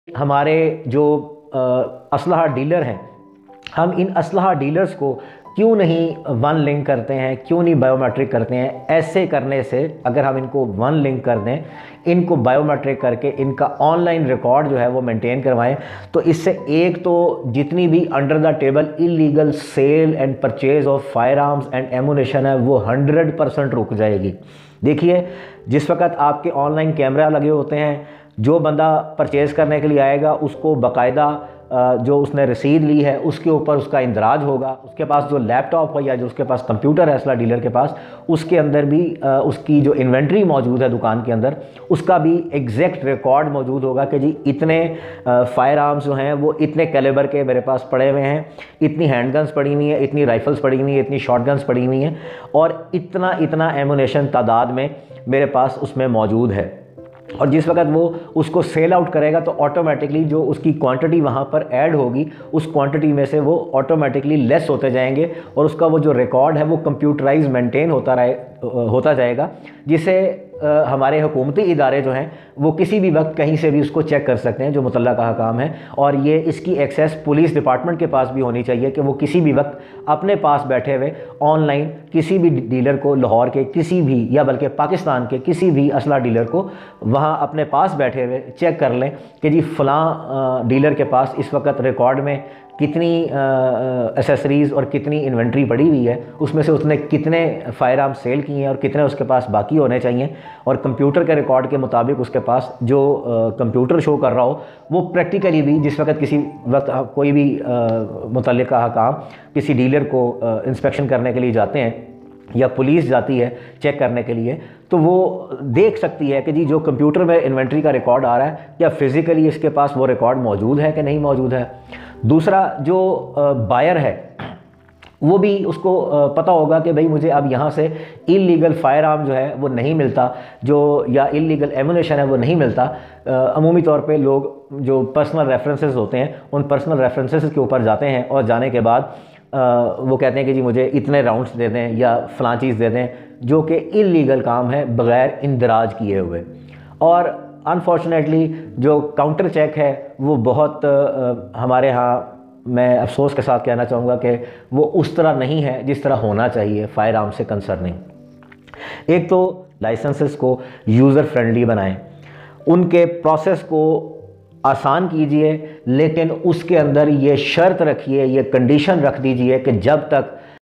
How To Control Illegal Selling & Misuse Of Firearms
Important Lecture On The Topic Of Illegal Selling Of Firearms, Gun Control, Misuse Of Weapons In Pakistan And Arms Licences Policy. Humble Suggestions For All Concerned Policy Makers, Police & LEA's Of Government Of Pakistan 🇵🇰

For. Customized Kydex Holsters
Contact Patriot Gear